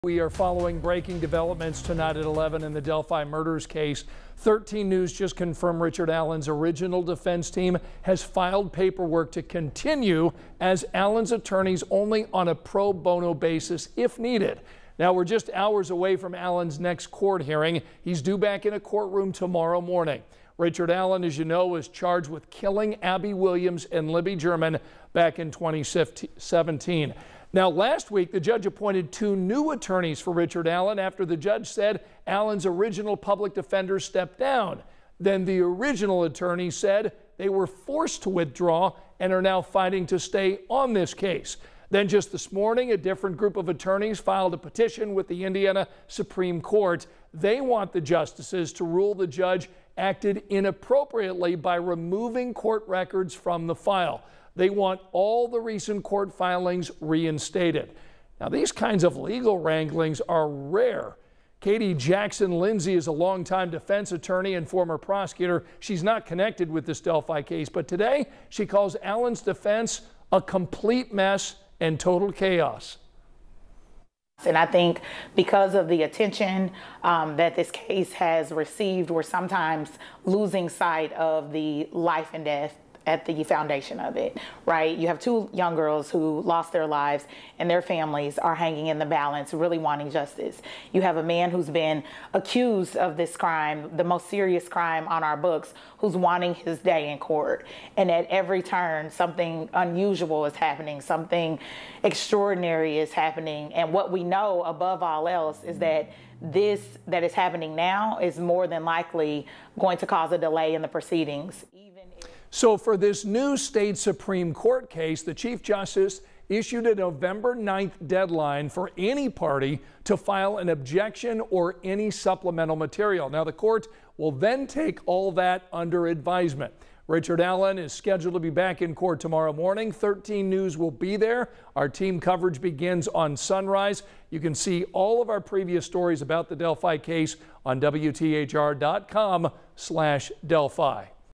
Richard Allen's original lawyers file to work pro bono
Last week, a judge appointed two new attorneys to represent Richard Allen.